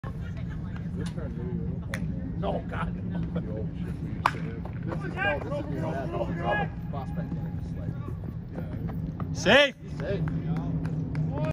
Infield Single to First
Sparks North - speed down line makes routine play a single.